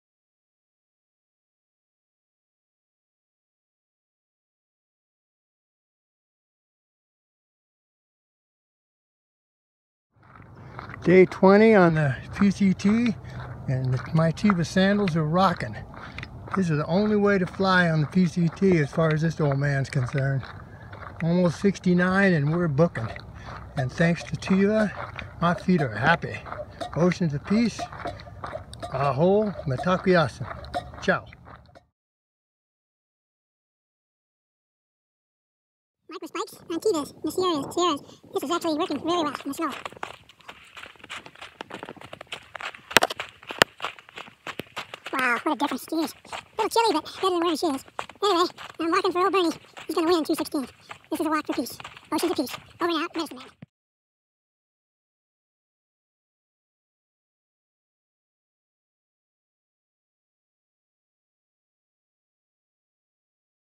Tevas With Microspikes on the Pacific Crest Trail
Day 62 , mile 909 on the Pacific Crest Trail with Dr Stephen Blake, trail name Medicine Man showing how you can walk the PCT barefoot and with Tevas please share 🙏